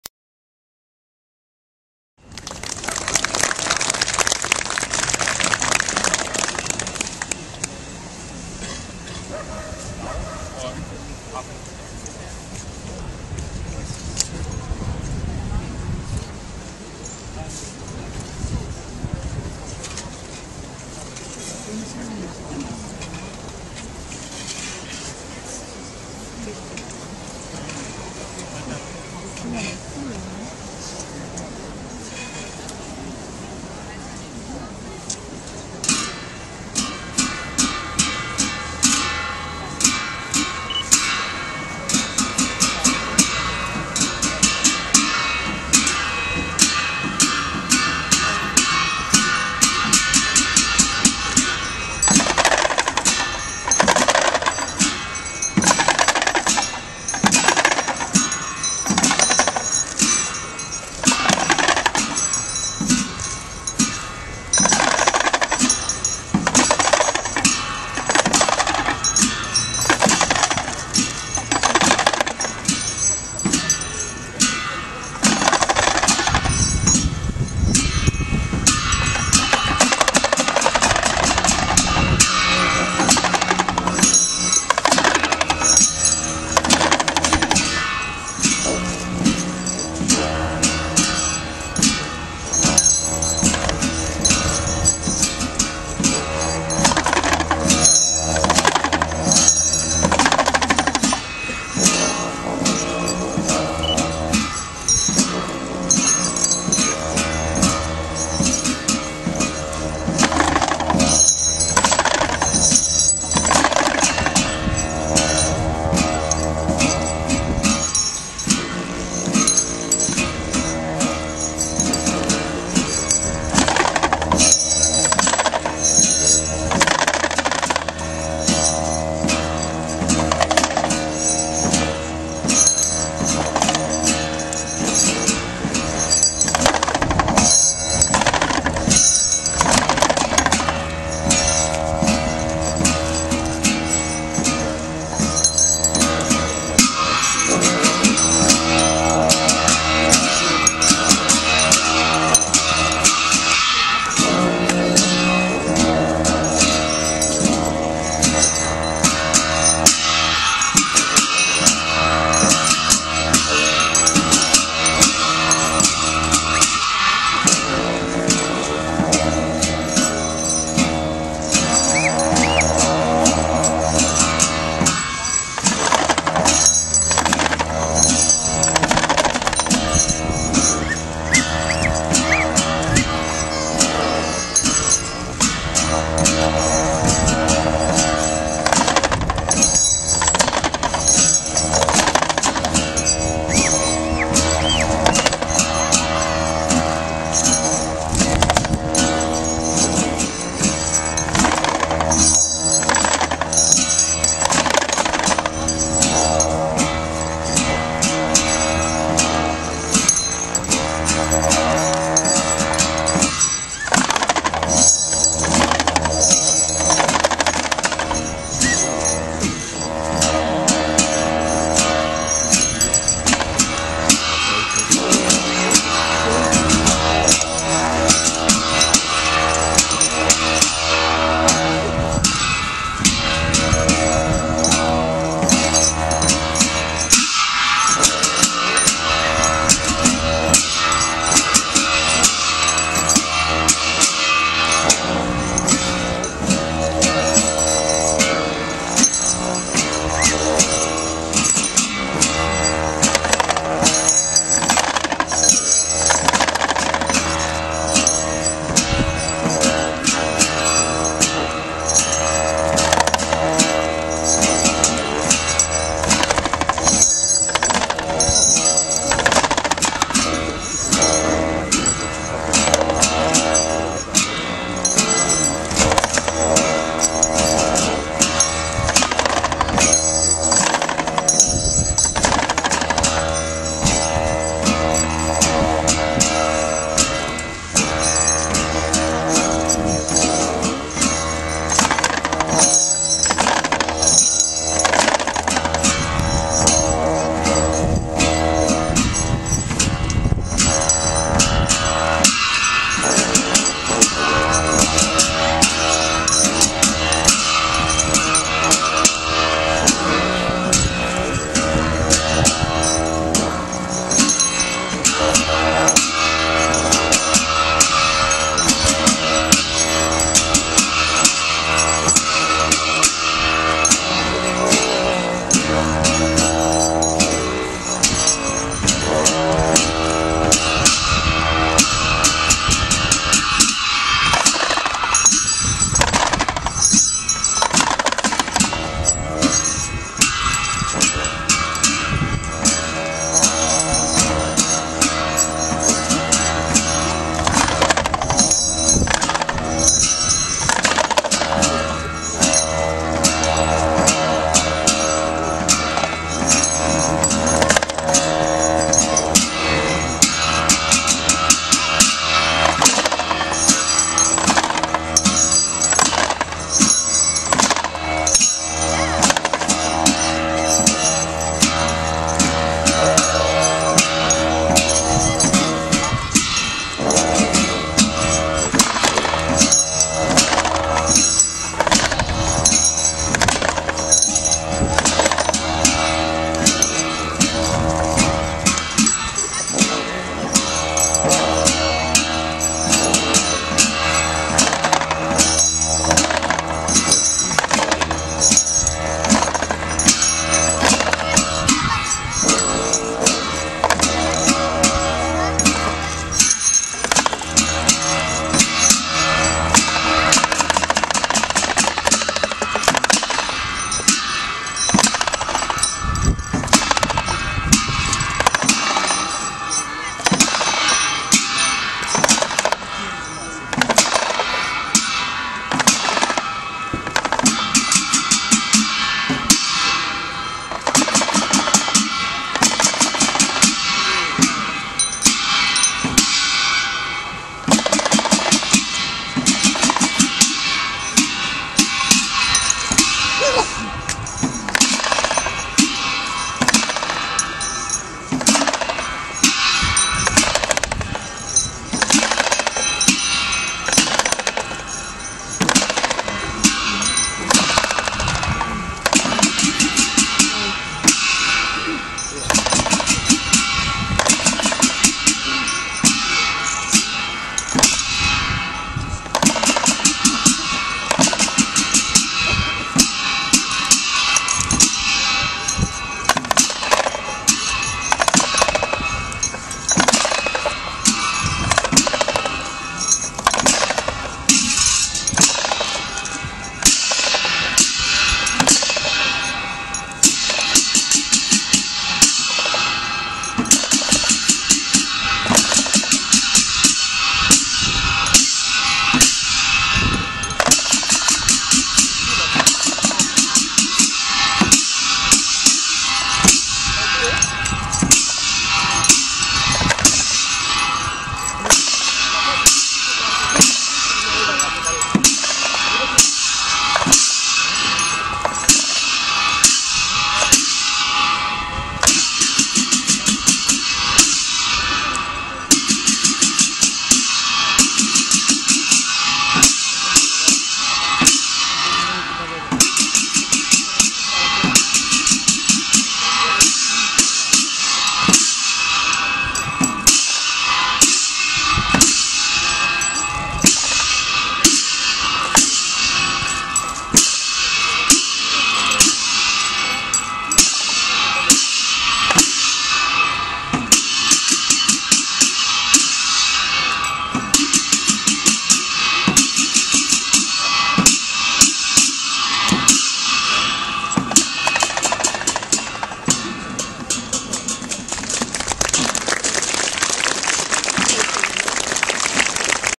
Pawo Dance
Buddhist Monks from Bhutan perform Pawo Dance. Cologne / Germany, May 17th 2010